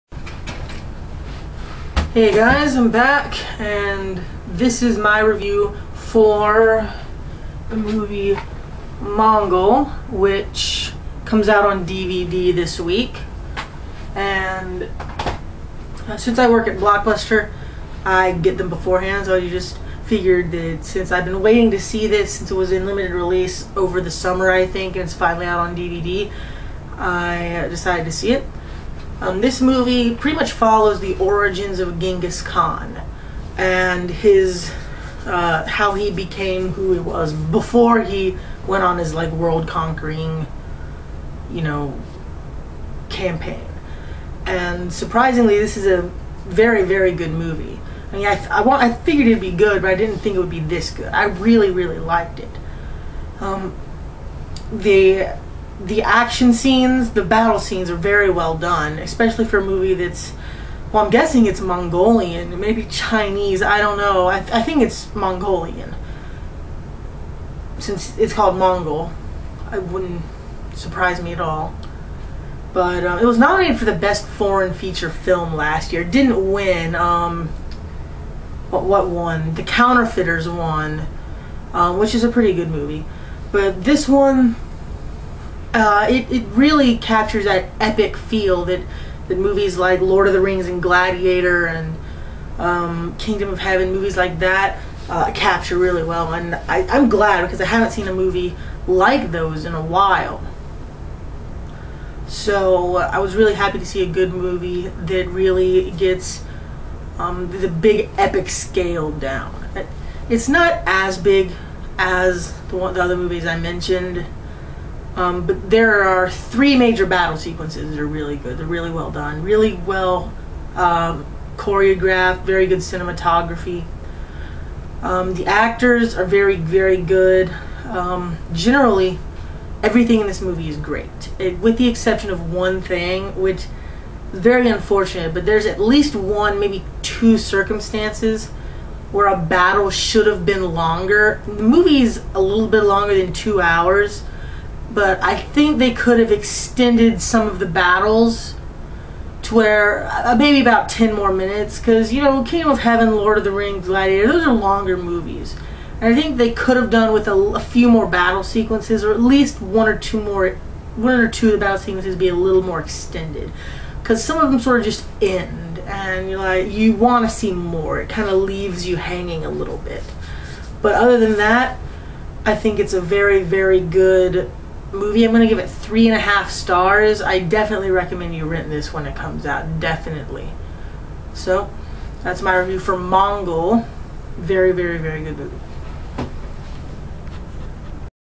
Mongol review
Mongol is an epic war film chronicling the early days of Genghis Khan. This film gets 3 and 1/2 stars.